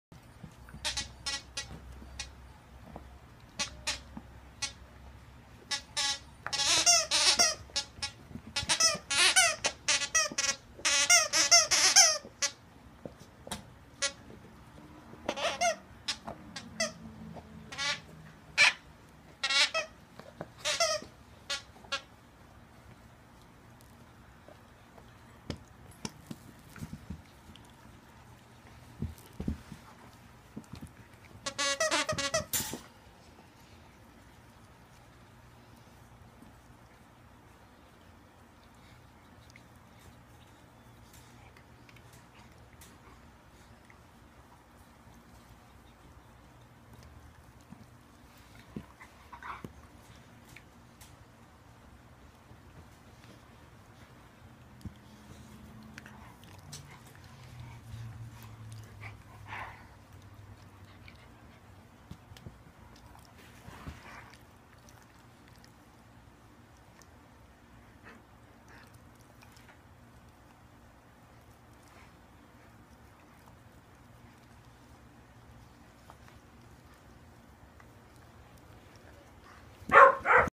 Ibizan hound and whippet puppies, gentle play
Very sweet Ibizan hound and 2 whippet puppies playing